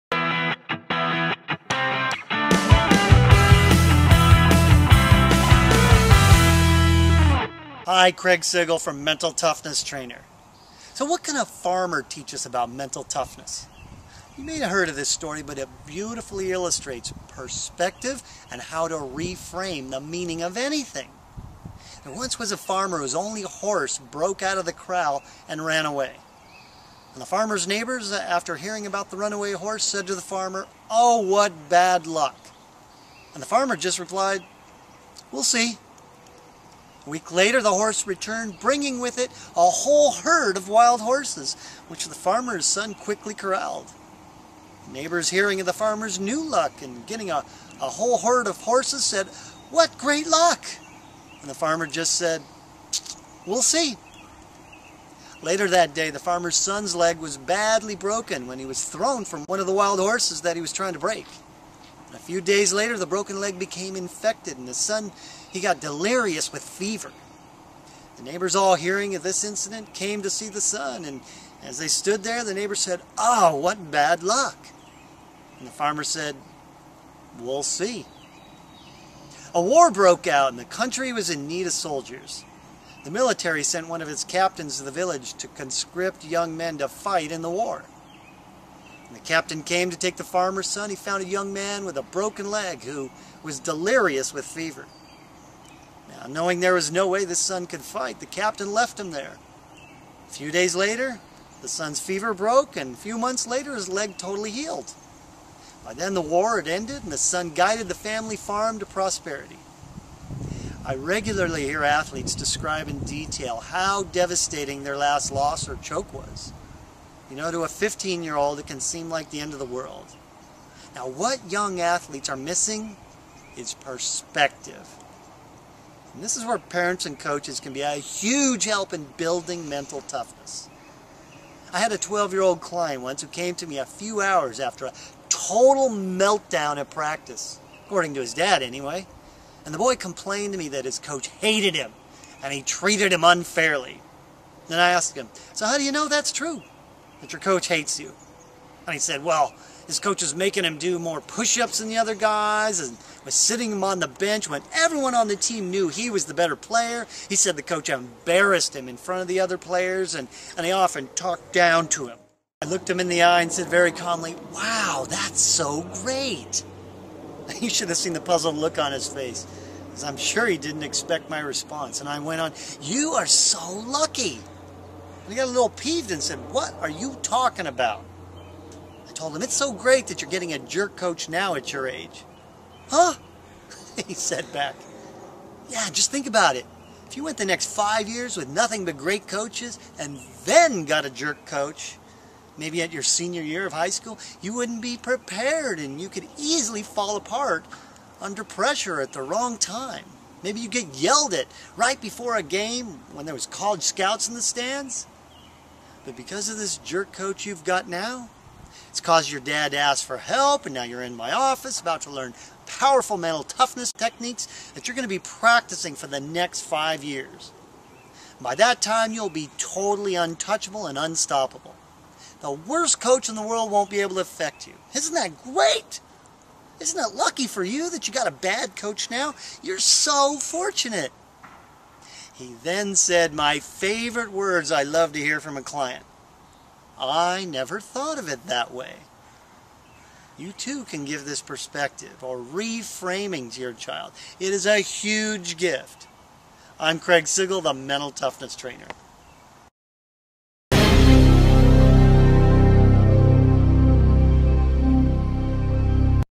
Develop Self Confidence In Youth Sports - Craig Sigl
What can a farmer teach us about mental toughness?

You may have heard of this story but it beautifully illustrates perspective and how to reframe the meaning of anything...

There once was a farmer whose only horse broke out of the corral and ran away. The farmer's neighbors, upon hearing of the run away horse said to the farmer, "Oh what bad luck!"

The farmer just replied, "We'll see."

About a week later, the horse returned, bringing with it a whole herd of wild horses, which the farmer and his son quickly corralled.

The neighbors, hearing of the farmer's new luck getting a herd of horses said, "Oh what good luck!" The farmer just replied, "We'll see."

Later that day, the farmer's son's leg was badly broken when he was thrown from one of the wild horses he was trying to break.

A few days later the broken leg became infected and the son became delirious with fever. The neighbors, all-hearing of the incident, came to see the son. As they stood there, the neighbors said, "Oh what bad luck!" The farmer replied, "We'll see."

A war broke out and the country was in need of soldiers.  The military sent one of its captains to the village to conscript young men to fight in the war.

When the captain came to take the farmer's son he found a young man with a broken leg who was delirious with fever. Knowing there was no way the son could fight, the captain left him there.

A few days later, the son's fever broke and a few months later, his leg healed.  By then the war had ended and the son guided the family farm to prosperity.

I regularly hear athletes describe in detail how devastating their last loss or choke was. To a 15 year old, it can seem like the end of the world.  What young athletes are missing is...

Perspective

This is where parents and coaches can be a huge help in building mental toughness.

I had a 12-year-old client come to me a few hours after a total meltdown at practice (according to his dad).  The boy complained to me that his coach hated him and treated him unfairly.

I asked, "How do you know this is true?"

He said his coach was making him do more pushups than the other guys and was sitting him on the bench when everyone on the team knew he was the better player.  He said the coach embarrassed him in front of the other players and often talked down to him.

I looked him in the eye and said very calmly "Wow, that's so great!"

You should have seen the puzzled look on his face, because I am sure he did not expect my response. I went on, " You are so lucky!"

Now he got a little peeved and said "What are you talking about?"

I told him "It's so great that you are getting a jerk coach now at your age."

"Huh?" he replied.

"Yeah, just think about it...if you went the next 5 years with nothing but great coaches and THEN got a jerk coach your senior year of high school, you wouldn't be prepared and could easily fall apart under pressure. Maybe you'd get yelled at right before a game when there was college scouts in the stands. "

"But, because of this jerk coach you've got, it caused your dad to ask for help and now you're in my office about to learn powerful mental toughness techniques that you will be practicing for the next 5 years."

"By that time, you will be totally untouchable and unstoppable. The worst coach in the world won't be able to affect you".

"Isn't that great! Isn't it lucky for you that you got a bad coach now? You are so fortunate..."

He then said my favorite words I love to hear from a client:  "I never thought of it that way".

You can too can give this perspective or reframing to your child.  It is a huge gift!

and  "Master The Pressure" a GAME-CHANGING guided visualization for teen athletes.